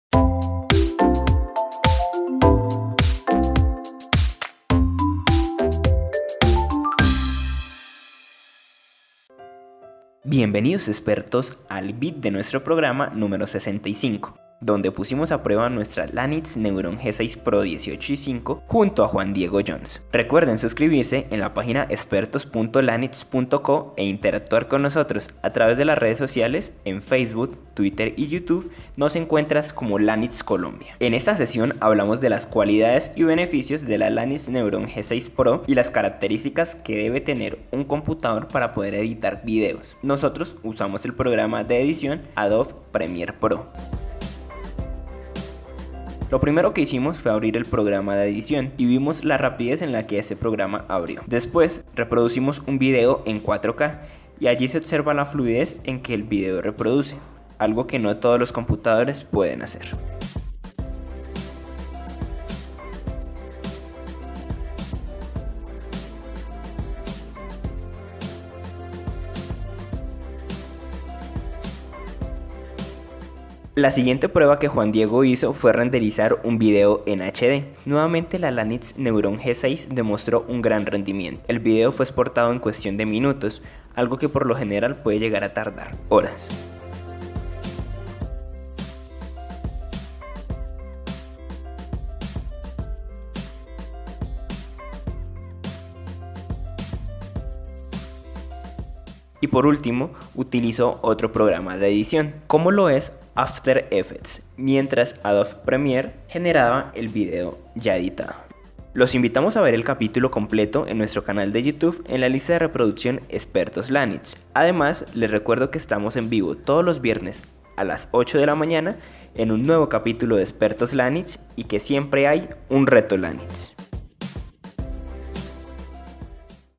[BITS] ExpertosLanix 65: Editando Video con Lanix Neuron G6 (2018) ft. Juan Diego Johns
Bits ExpertosLanix 65, Junto a nuestro gran invitado Juan Diego Johns hablamos de las características que debe tener un computador para poder editar videos. Con base a esto pusimos a prueba la Lanix Neuron G6 - B18i5 utilizando el programa de edición Adode Premiere Pro para reproducir un video que está en 4k y luego renderizarlo.